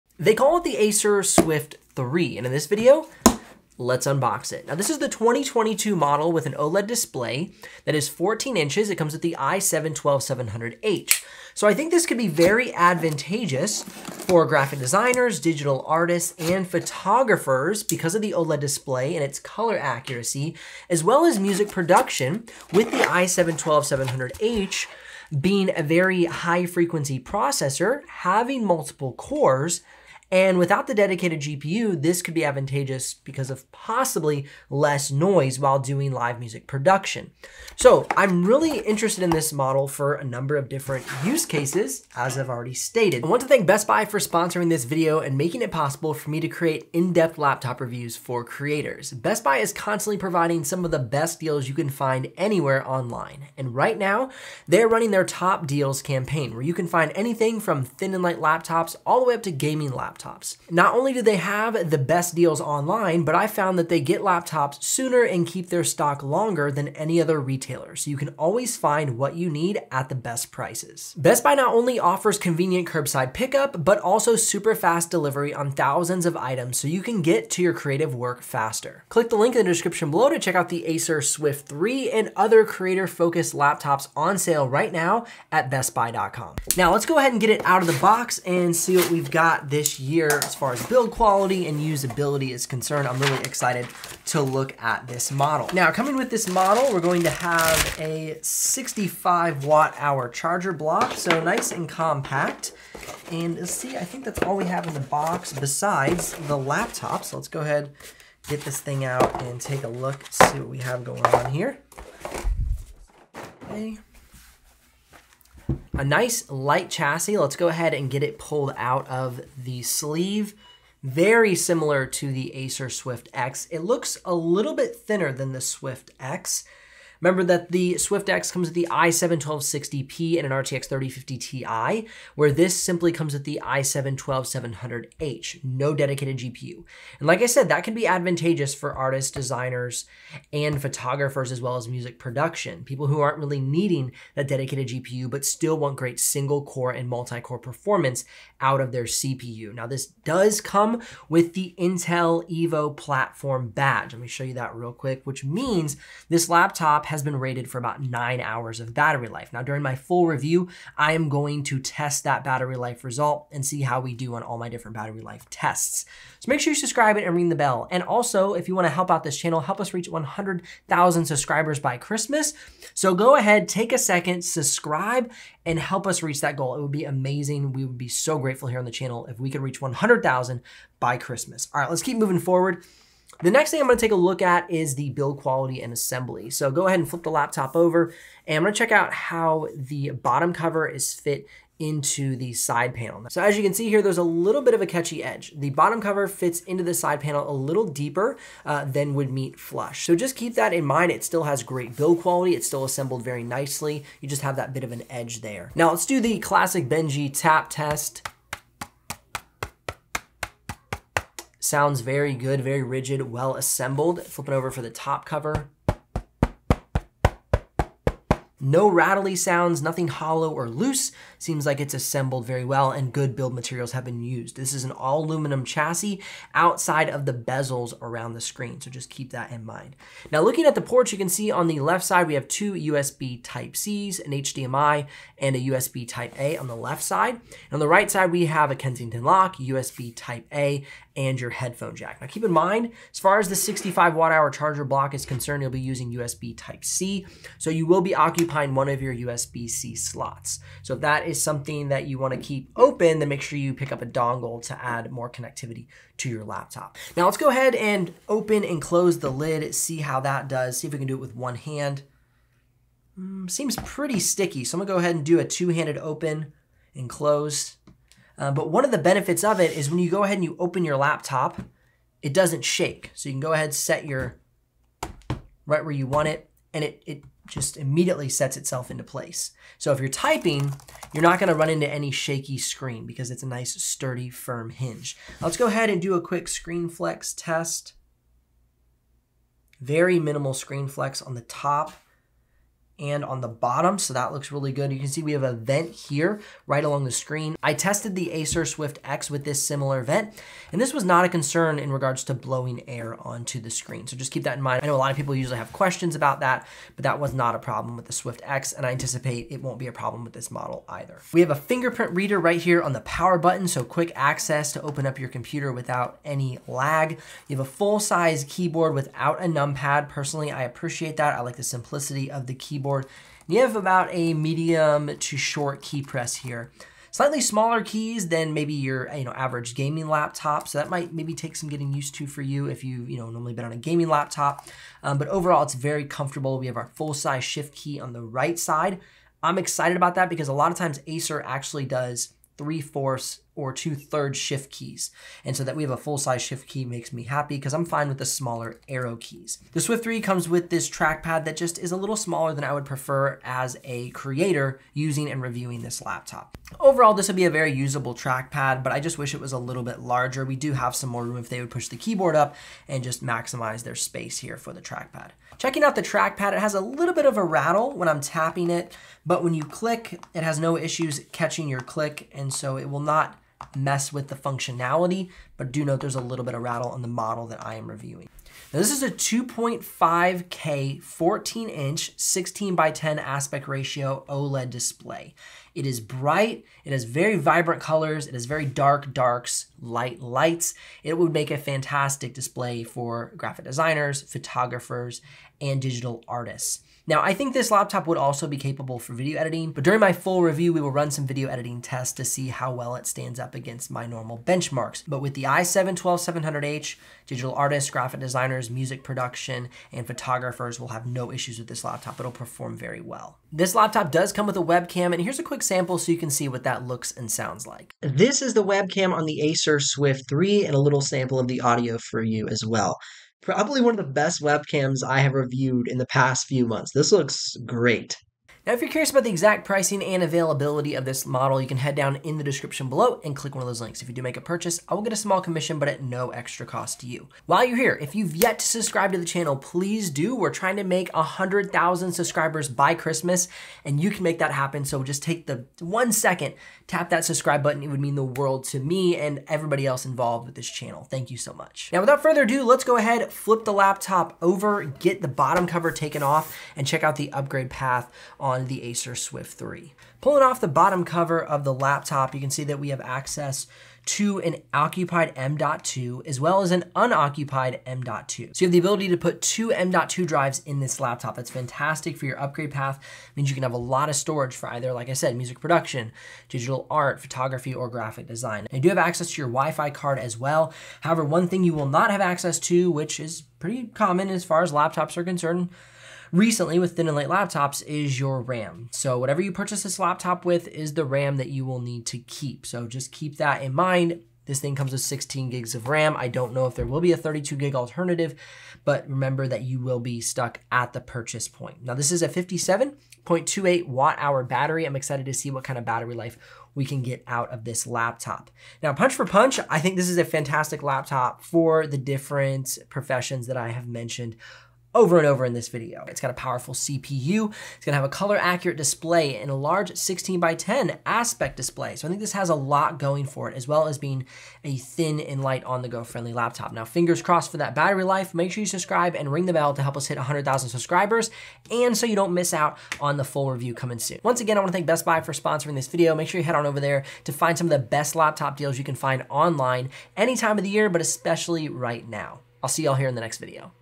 What's New in the Acer Swift 3 OLED // Unboxing
2022 Acer Swift 3 OLED Unboxing. I'd like to thank Best Buy for sponsoring this video ad and providing some amazing sales on so many fantastic laptops for Creative Professionals. 🎆 Best Buy Top Deals

💻 Acer Swift 3 OLED - Commission Earned )

The Acer Swift 3 just became a huge contender for graphic designers, digital artists, photographers, and music production. With its i7-12700H and 14-inch OLED display it has a lot to offer this year!

Timestamps:
0:00 - Unboxing the HP Envy 16
0:43 - Best Buy Sale happening right now!
1:26 - What's in the box?
1:35 - 65W Power Supply
1:54 - Pulling the laptop out of the sleeve
2:04 - Weight & Thickness
3:03 - Build Quality Assessment
3:25 - Tap Test
3:50 - I/O Ports
4:16 - Opening the Lid
4:45 - Screen Wobble & Screen Flex
4:54 - Is the vent on the keyboard deck a thermal concern?
5:10 - Checking out the keyboard
5:49 - Trackpad impressions
6:18 - What I love about this laptop
6:53 - Webcam is AMAZING
7:32 - Can you upgrade the 2022 Acer Swift 3 OLED?
7:42 - You can upgrade the SSD on the Acer Swift 3 OLED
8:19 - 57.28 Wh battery
8:29 - Should you buy the Acer Swift 3 OLED?